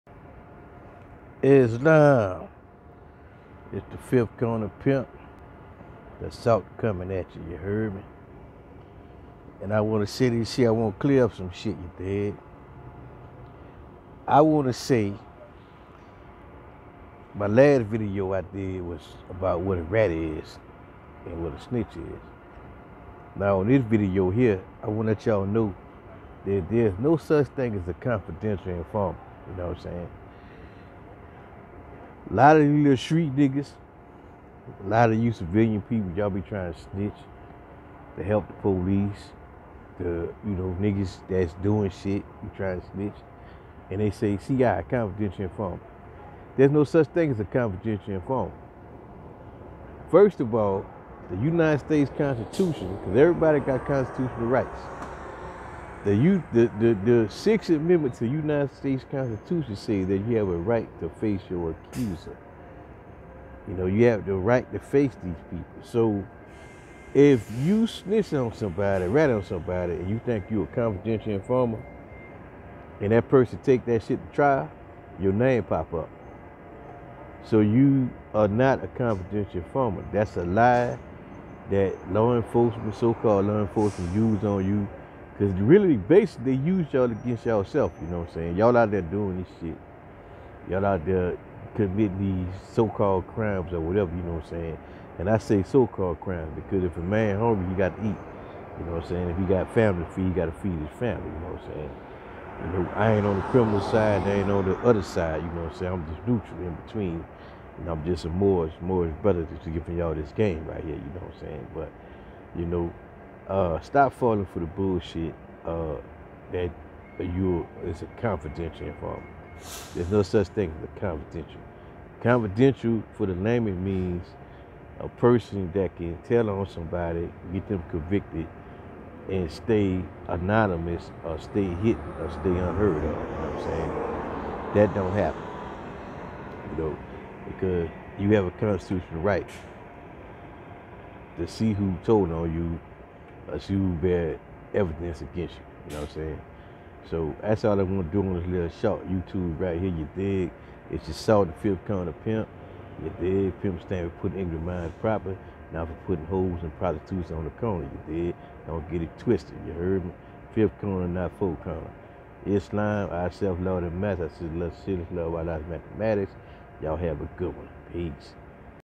United States constitution amendment 6 says there is no such thing as a confidential information.
Amendment VI

In all criminal prosecutions, the accused shall enjoy the right to a speedy and public trial, by an impartial jury of the state and district wherein the crime shall have been committed, which district shall have been previously ascertained by law, and to be informed of the nature and cause of the accusation; to be confronted with the witnesses against him; to have compulsory process for obtaining witnesses in his favor, and to have the assistance of counsel for his defense.